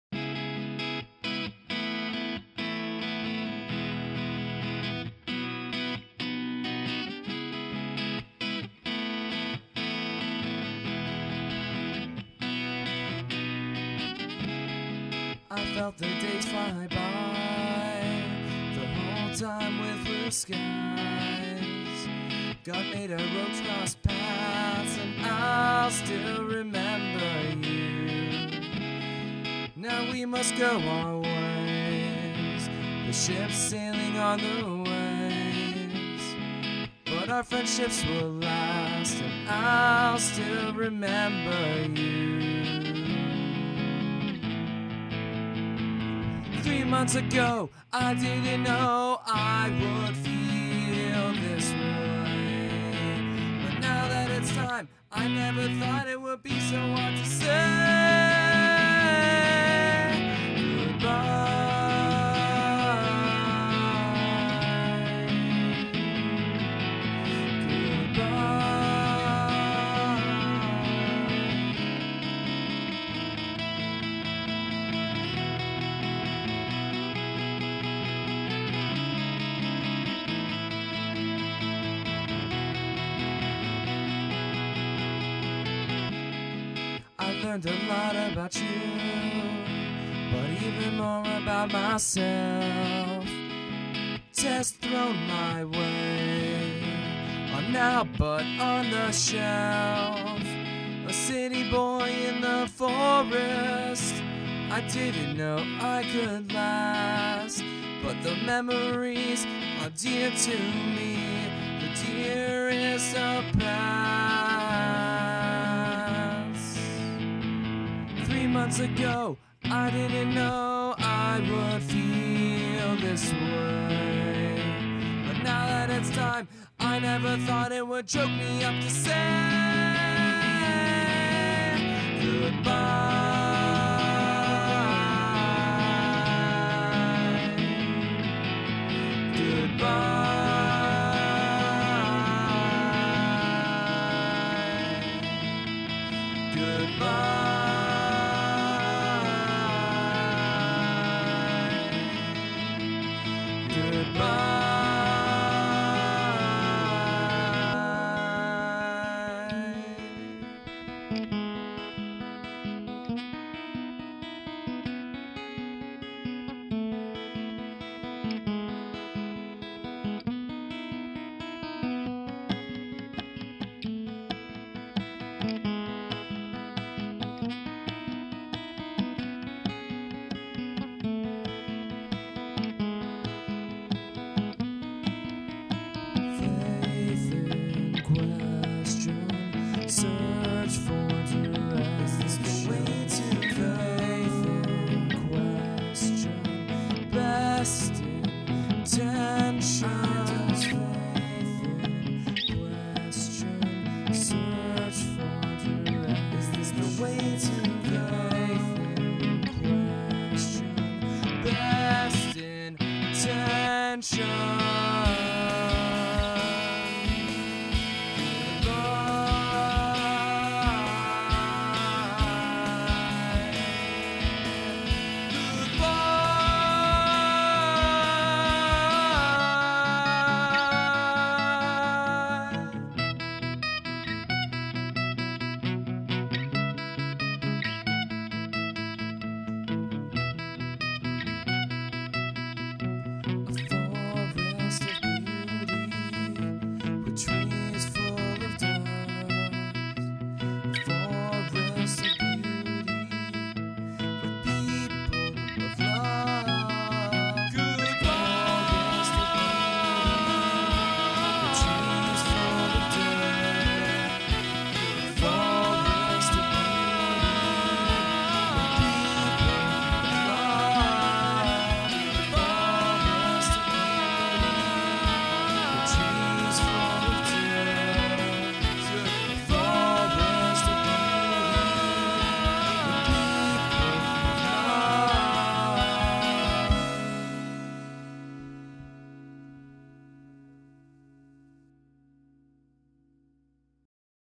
Bosch Summer Service '08
The song in the background is a song that a service youth, Kevin Ackermann, and I wrote and quickly recorded as a good-bye song for Bosch Baha'i School. Over the music I added a picture slideshow of my most memorable moments of serving at Bosch Baha'i School over the summer of '08.

Lyrics: I felt the days fly by
the whole time with blue skys
God made our roads cross paths
and i'll still remember you

Now we must go our ways
the ships sailing on the waves
but our friendships will last

3 months ago
I didn't know
I would feel this way
Now that it's time
i never thought
it would be so hard to say

Good-bye
Good-bye

I learned a lot about you
but even more about myself
tests thrown my way
are now but on the shelf

a city boy in the forest
i didn't know i could last
but the memories are dear to me,
the dearest of pasts

3 months ago
I didn't know
i never thought
it would choke me up to say

Good-bye
Good-bye

Faith in question
Search for direction
Faith in question
best intentions

a forest of beauty
with trees full of doves
with people of love